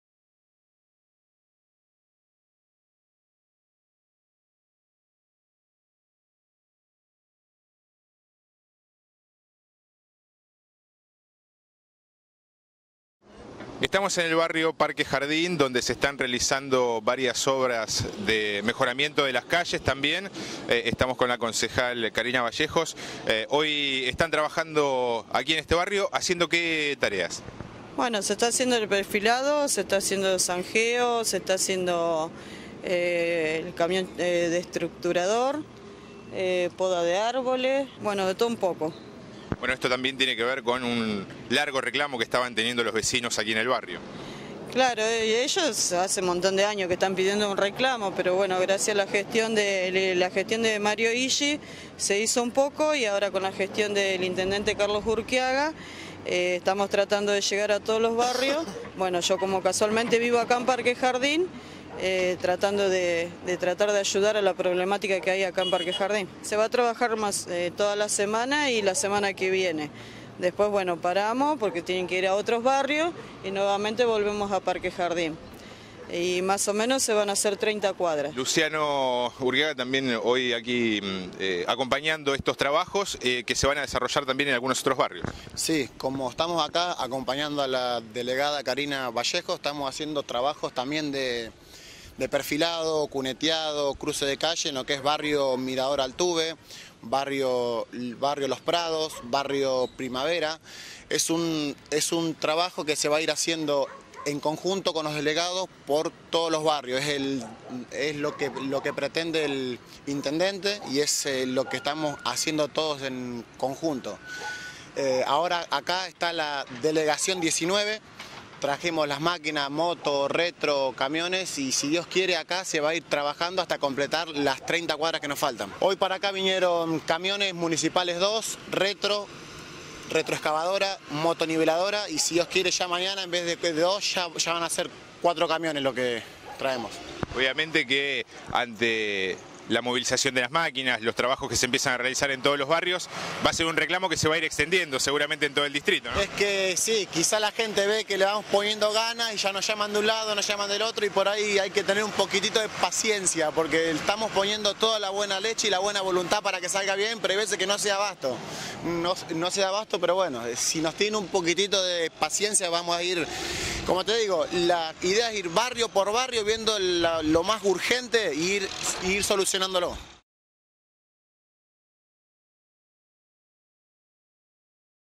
OBRAS EN BARRIO PARQUE JARDIN
Se están realizando obras de mejoramiento y limpieza en distintos barrios de José C. Paz, en este caso en el Barrio Parque Jardín.

Entrevistados: Karina Vallejos y Luciano Urquiaga